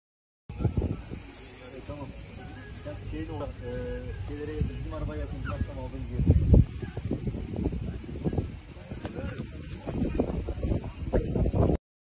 Ormanı ateşe vermeye çalışan kişi böyle görüntülendi
Bodrum'da iddiaya göre ormanlık alanda yangın çıkarmaya çalışan şahıs vatandaşlar tarafından görüntülendi.